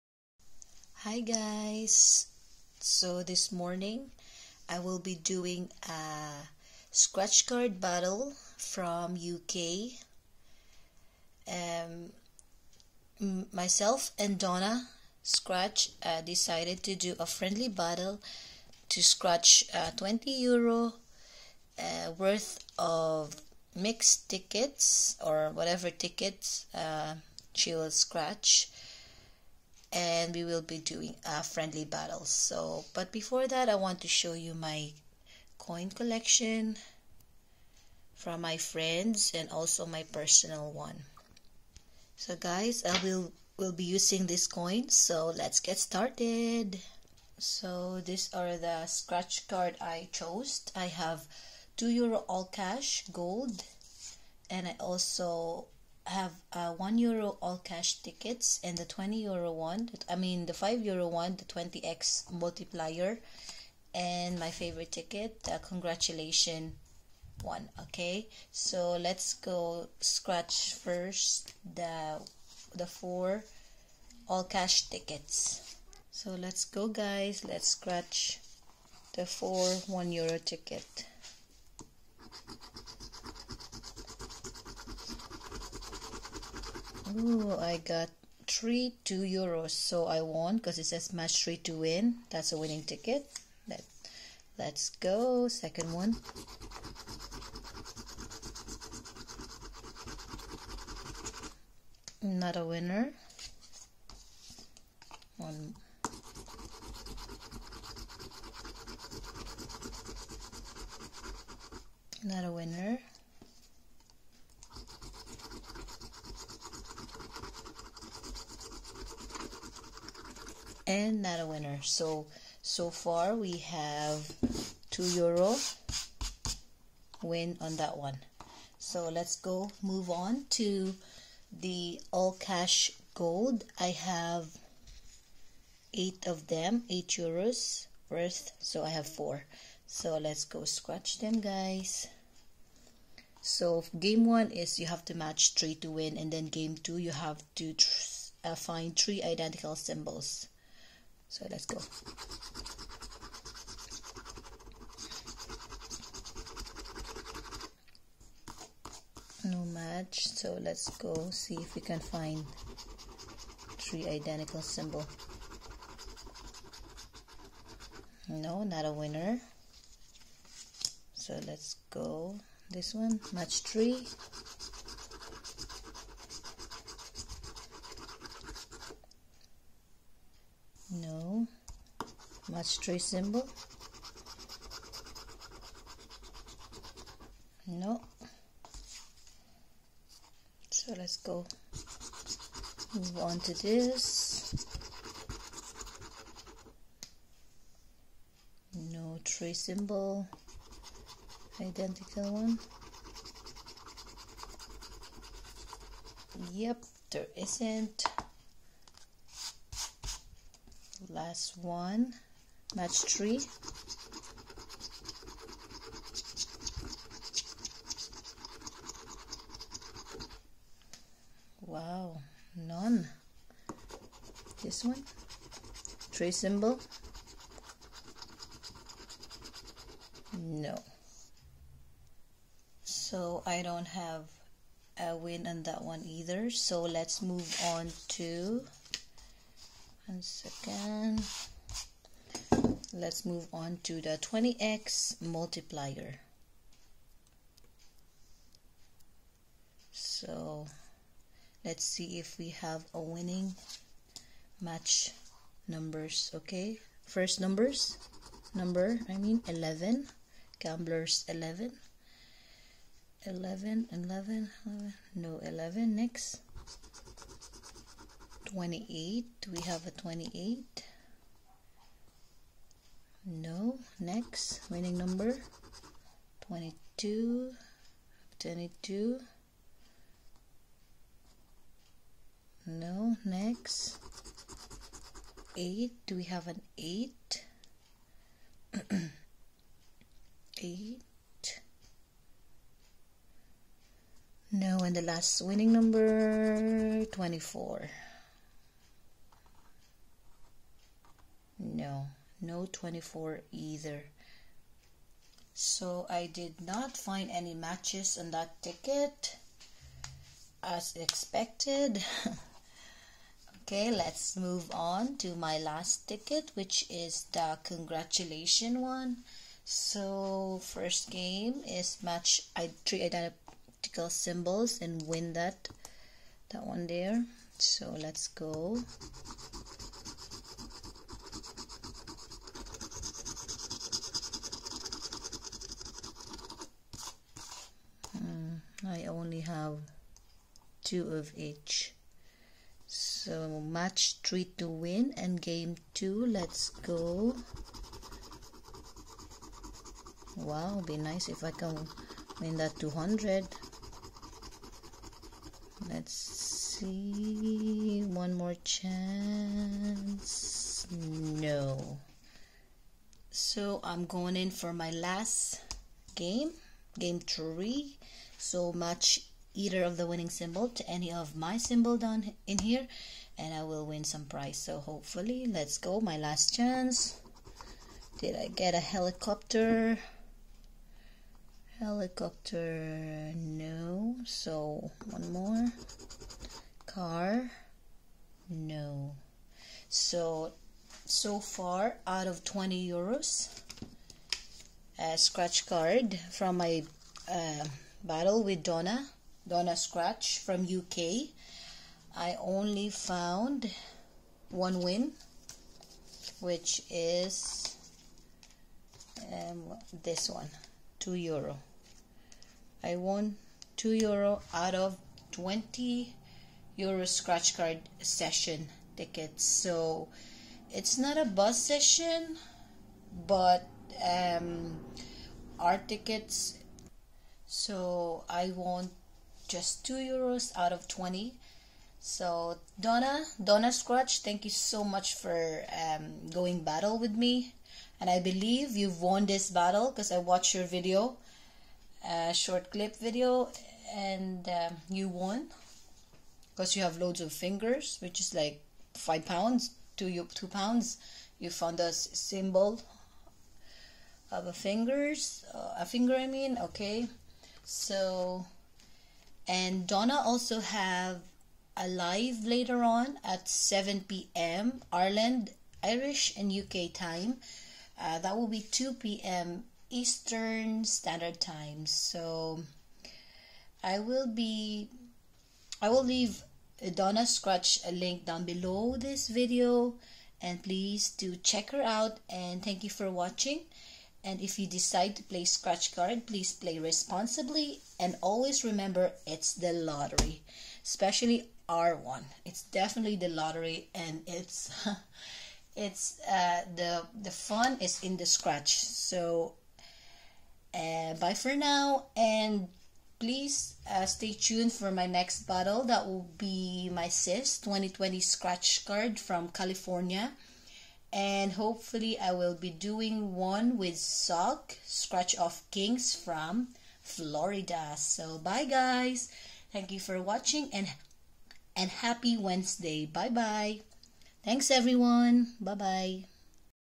IRELAND VS UK SCRATCHCARDS BATTLE! 🥳
FRIENDLY SCRATCHCARDS BATTLE WITH DONNA X SCRATCH FROM UK!

ENJOY SCRATCHING AND PLAYING AND PLS PLAY RESPONSIBLY!

MUCH LOVE AND LUCK!